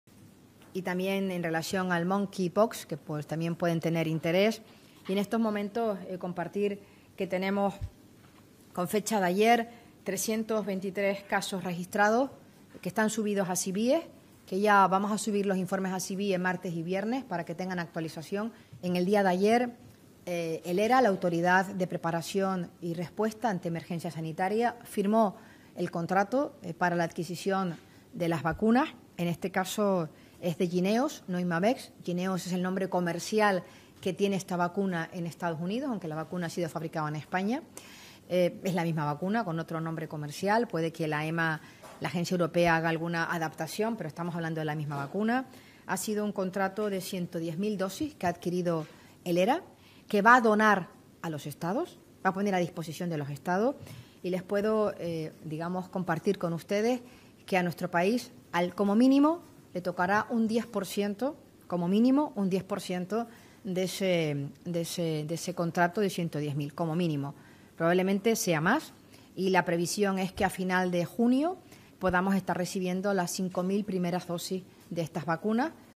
5.000 dosis contra la viruela del mono a finales de junio
Carolina Darias ha confirmado 323 casos de viruela del mono y ha dicho que, a finales de mes, llegarán 5.000 dosis de la vacuna contra ella. "Como mínimo, a España le va a tocar un 10% de las 110.000 inyecciones adquiridas por la Autoridad de Preparación y Respuesta ante Emergencias Sanitarias de la Comisión Europea (HERA)", ha asegurado la ministra.